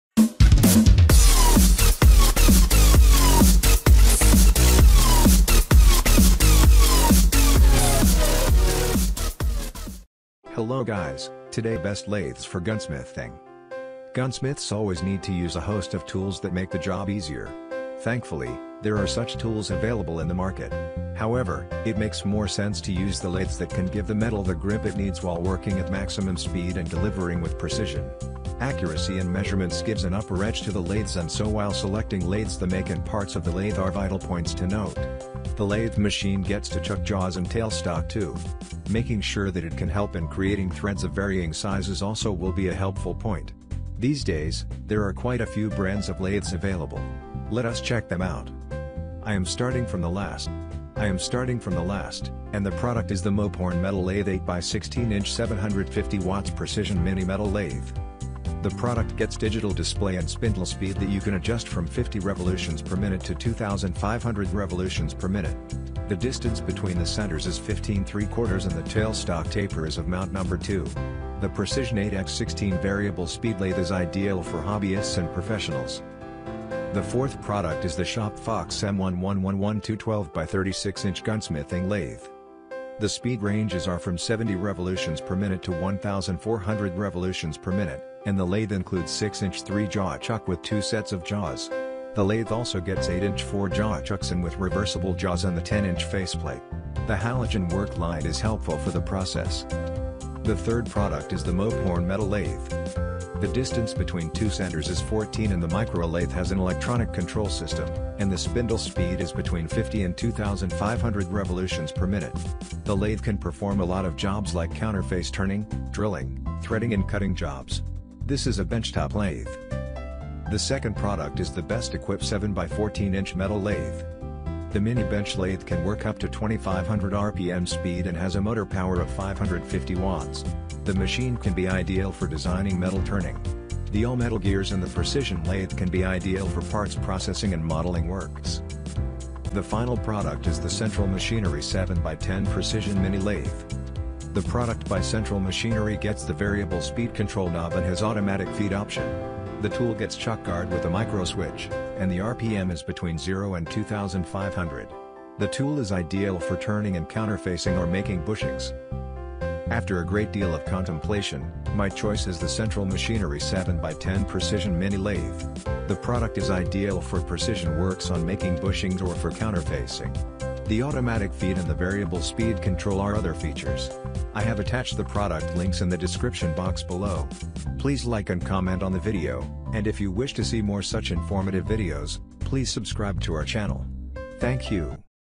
Best Gunsmith Lathe - Top List of 2021
Our Top Pick on Amazon (Amazon Link)

Here are the reviews of the lathe machines for gunsmithing. These are of top quality, and they have advanced features for precision turning.

Good Luck!

Top compared Best Gunsmith Lathe are:

1. Central Machinery 7X10 Precision Mini Lathe
2. BestEquip 7 x 14 inch Metal Lathe
3. Mophorn Metal Lathe
4. Shop Fox M11112 12x36 inch gunsmithing lathe
5. Mophorn Metal Lathe 8X16 inch